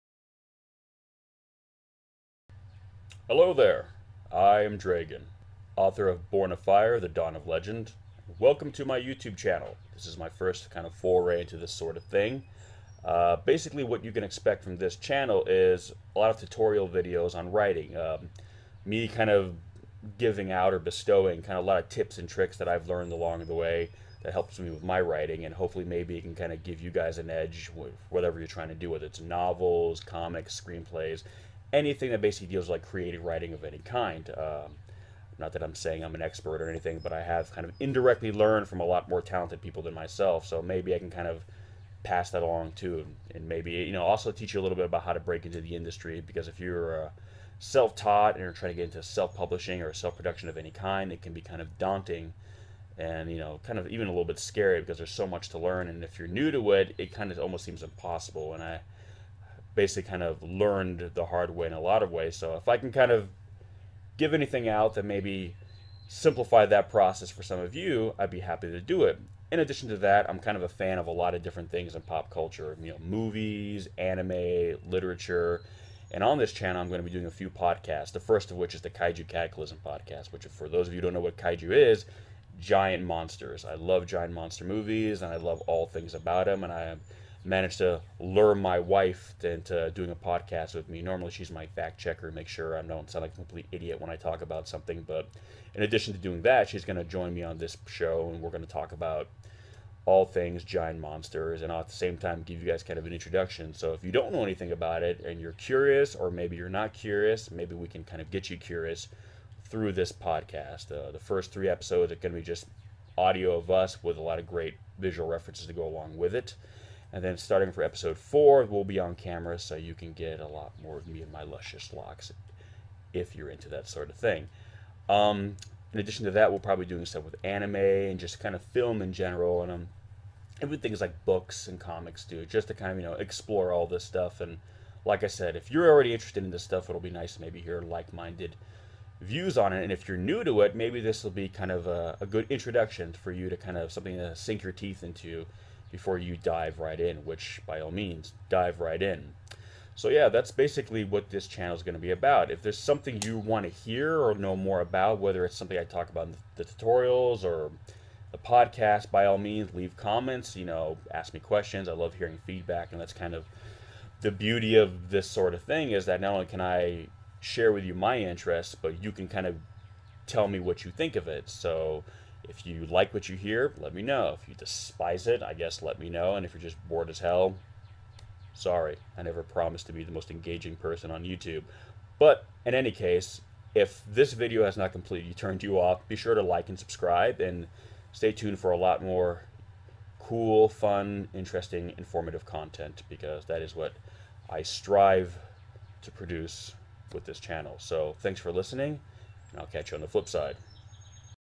Welcome to my channel
Introduction to my youtube channel and what to expect in the future.

BORN OF FIRE: THE DAWN OF LEGEND
AMAZON UK
AMAZON JP
Also available on iBooks